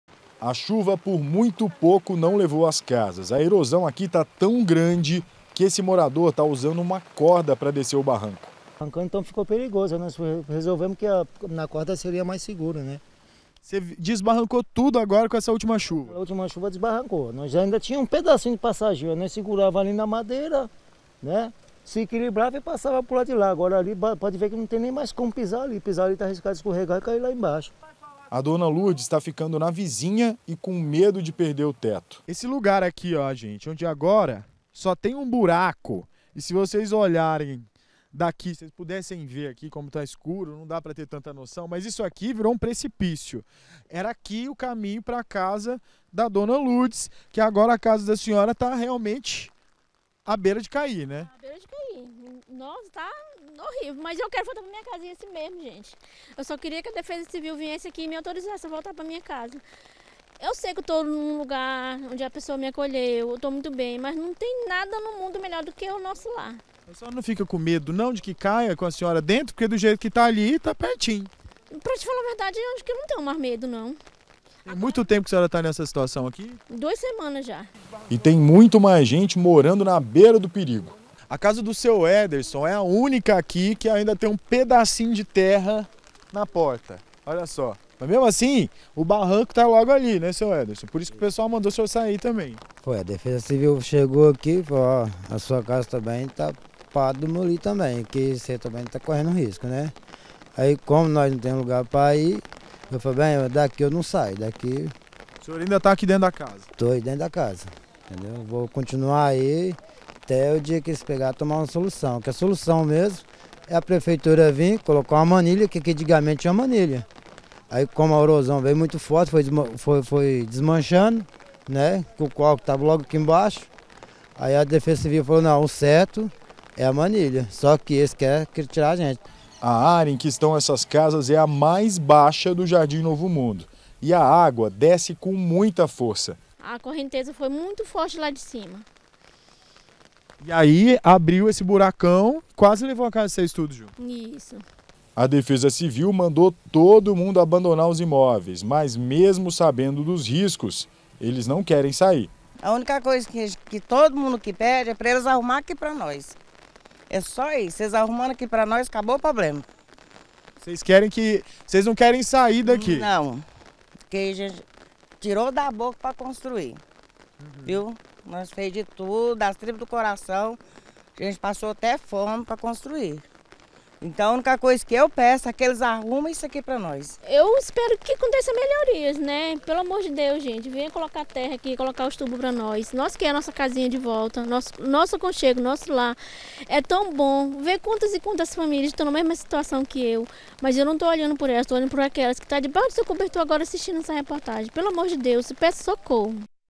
EROSÃO: CASAS PODEM CAIR DENTRO DO BURACO
Com a chuva de ontem, a erosão no Jardim Novo Mundo aumentou. O que tem tirado a paz de muitos. A área é considerada de risco pela defesa civil, e mesmo assim os moradores não querem deixar as casas.